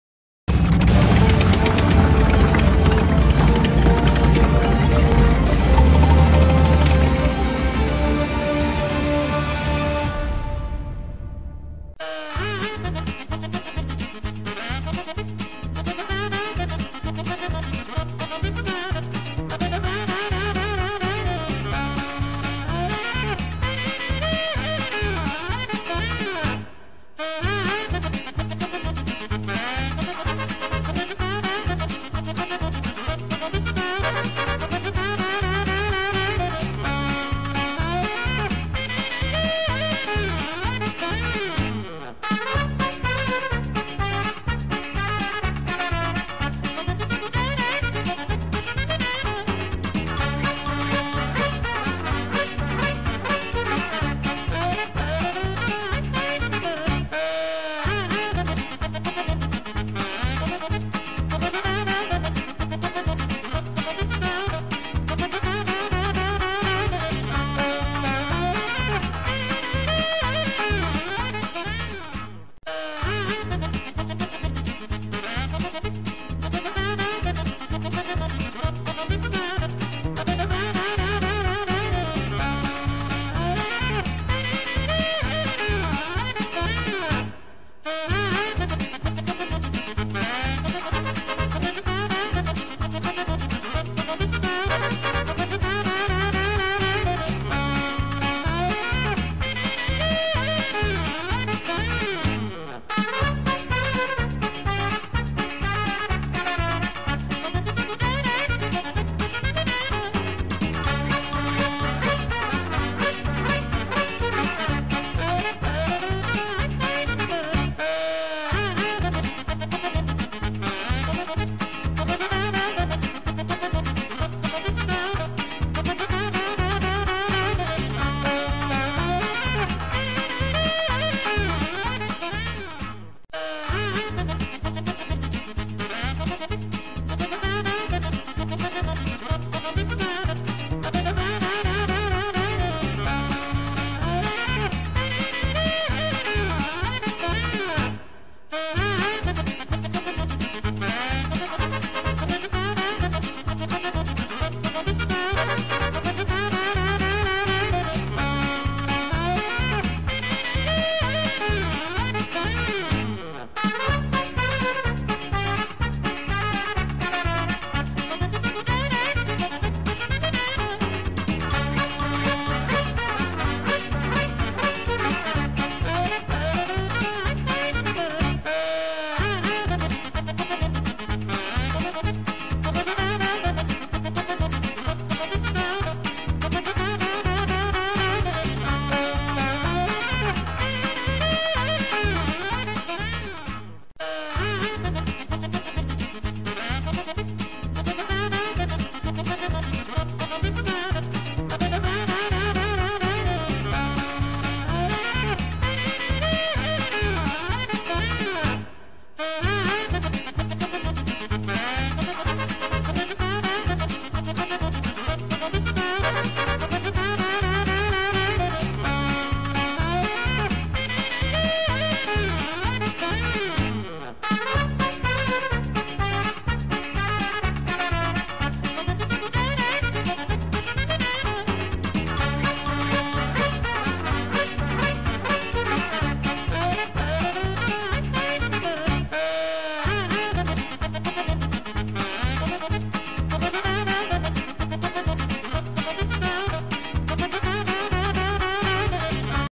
ELC EVEN 90 Ninja turkeys !!!
3 man Elc even 90 platoon in a t10 battle ! A lot of dip dive and dodging and will we win ???

A huge TY to tardis_who for my amazing intro